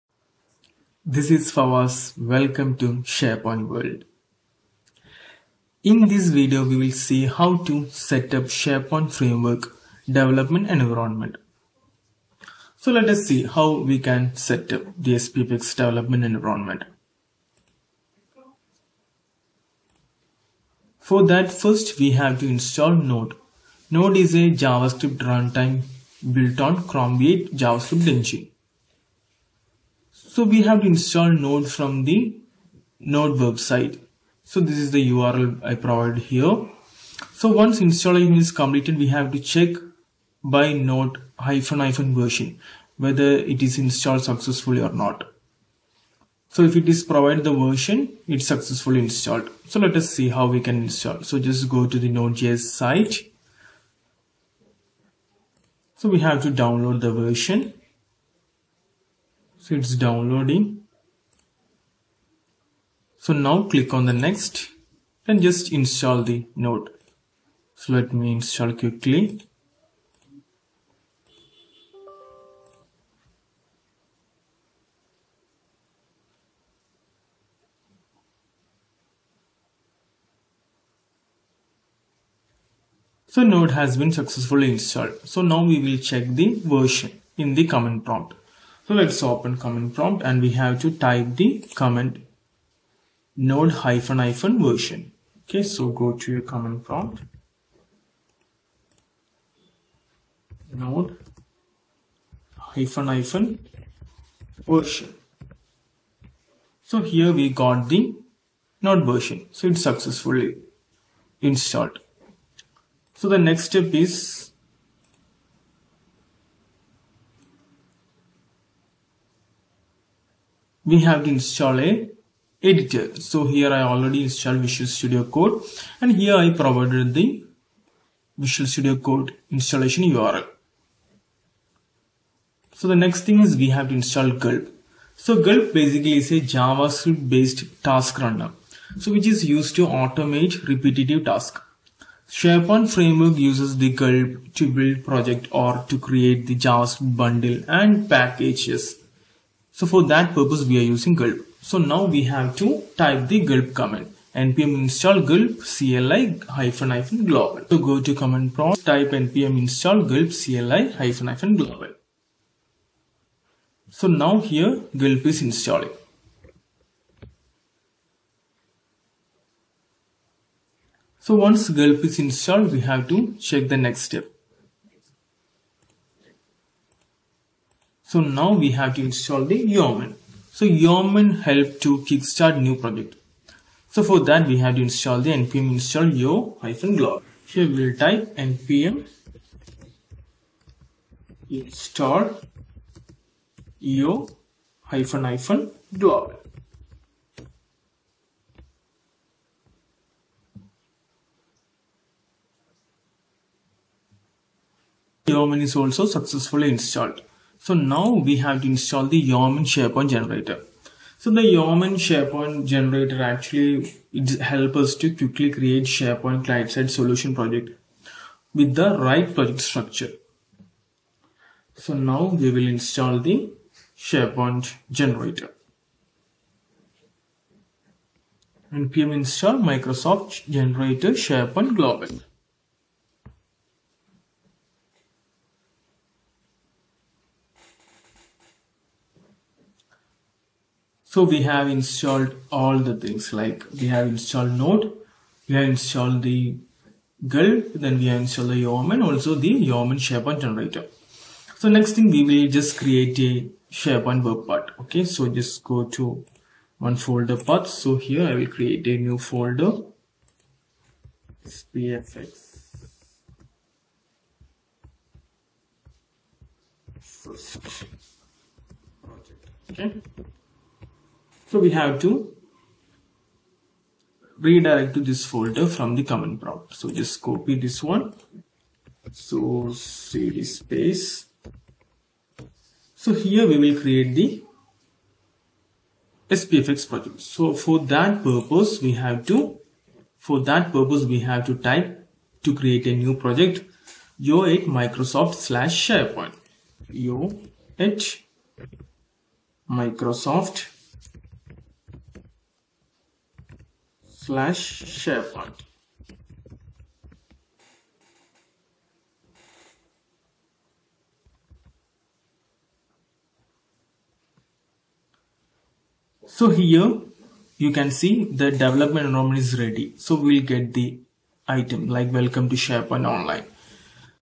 Set up your SharePoint Framework development environment(within 5 minute)
Install Node.js
Install Gulp
Install Yeoman
Install Yeoman SharePoint generator